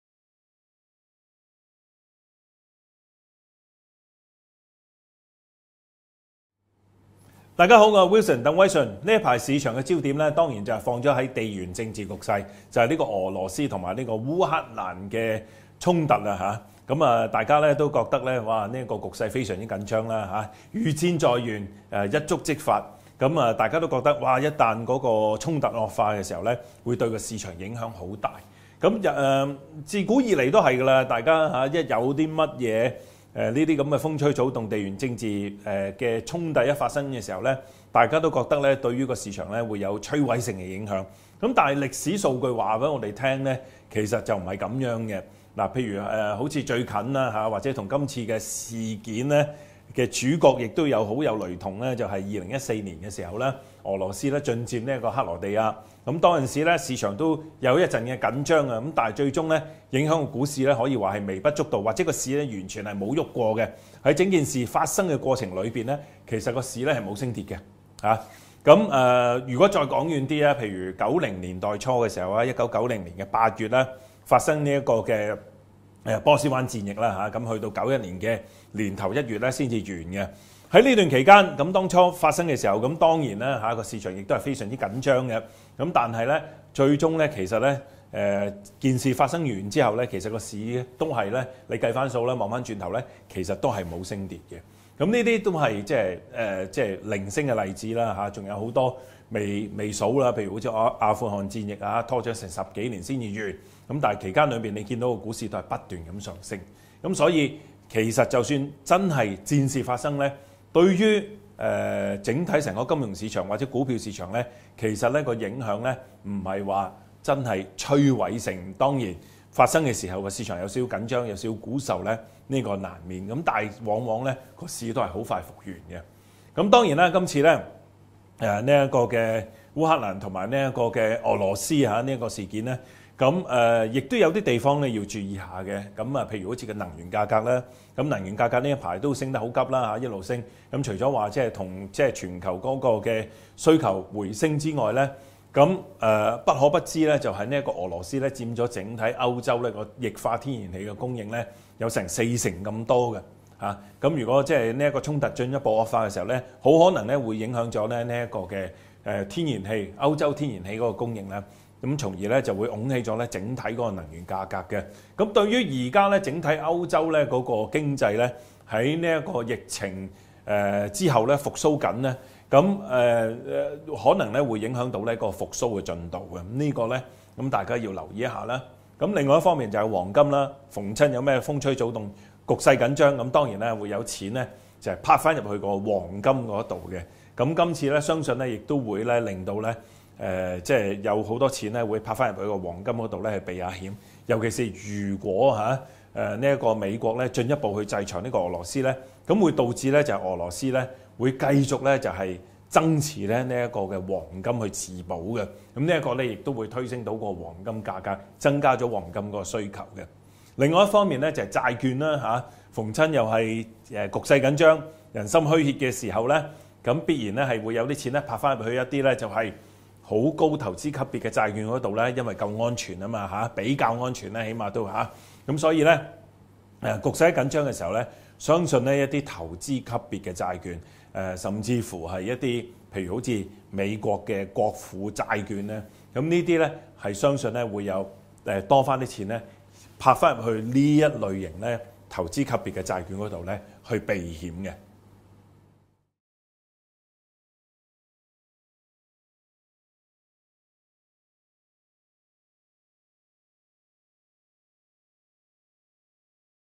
【星級觀點 – 地緣政治局勢對市場影響】
俄羅斯烏克蘭事件令局勢緊張，究竟從歷史數據顯示，衝突事件會否對投資市場帶來毁滅性影響？於局勢不穩時候，投資時又應留意哪一方面資訊？

市場瞬息萬變想參考投資意見？唔使四圍揾啦！DBS提供專業投資工具和專家意見，幫您掌握投資優勢。